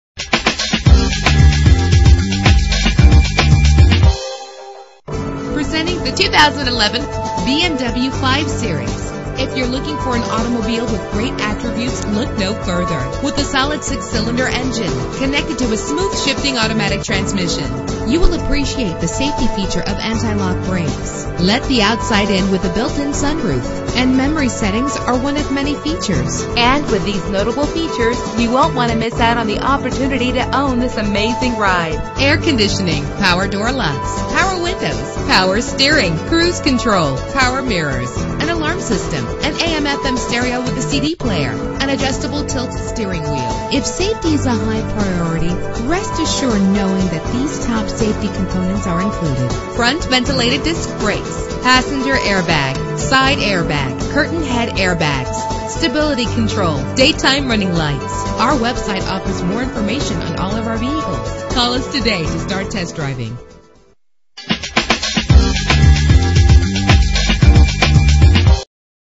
2011 BMW 5 Series - Athens Ga
Year: 2011
Make: BMW
Model: 5 Series
Trim: 535i xDrive
Engine: 3.0 liter inline 6 cylinder 24 valve
Transmission: 8-Speed Automatic
Color: Silver
Mileage: 0
Address:
3040 Atlanta Highway